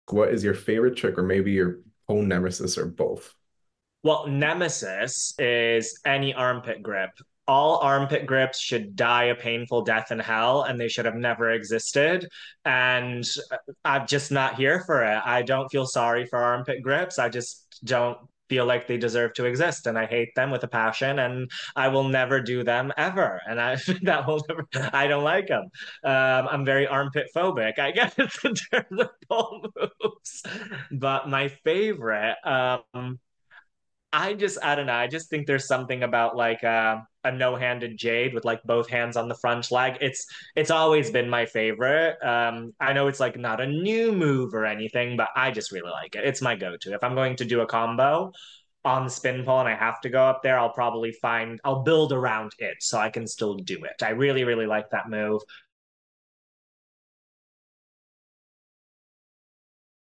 Jakub, The Pole Wizard Tells Us How He Feels About Armpit Holds
Have you checked out our new podcast interview with Pole Performer & Coach @the_pole_wizard

It’s available now! Listen wherever pods are cast or watch via our YouTube channel.

We learned so much about this incredible pole dancer’s life, including his hatred of armpit tricks, as described in this clip 😂😂😂

Are you teamarmpit or do you agree with Jakub?

And share with a friend who may enjoy ❤️